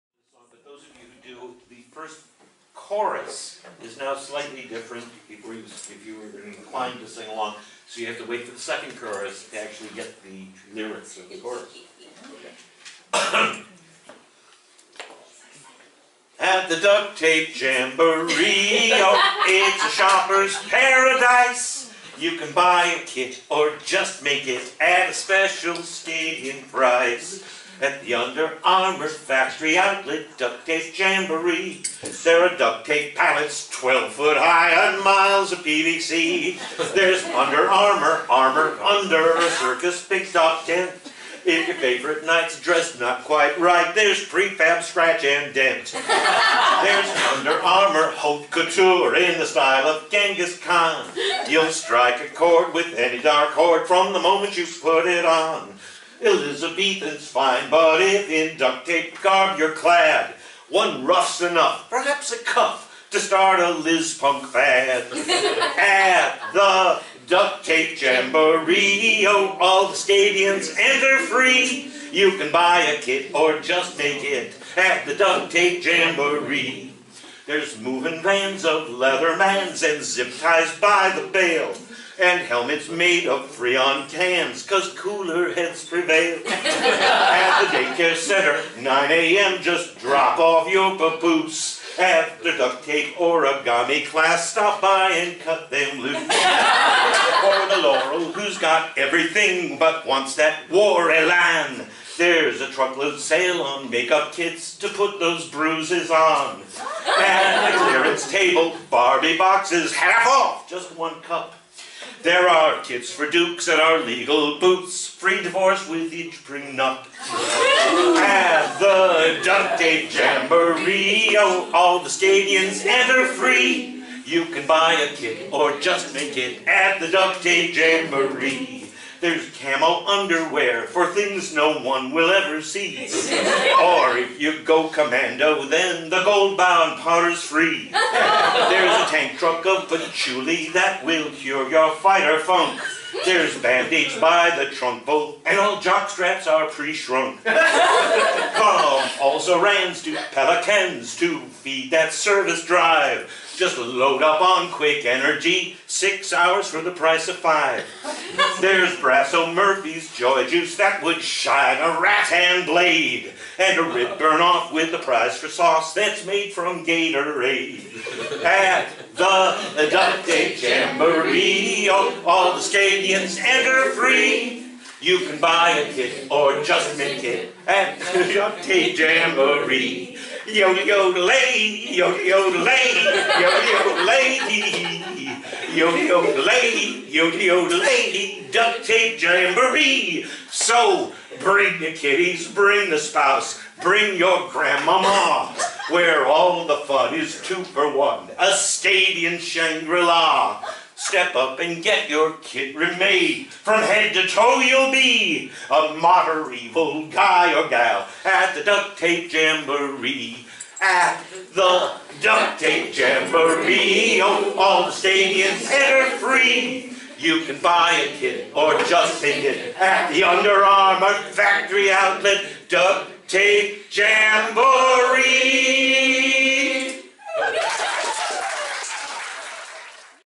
Llywelyn Glyndwr: Concert
Recorded at Midrealm’s Bardic Madness XVI, in the Shire of Ravenslake, 15 November, 2014.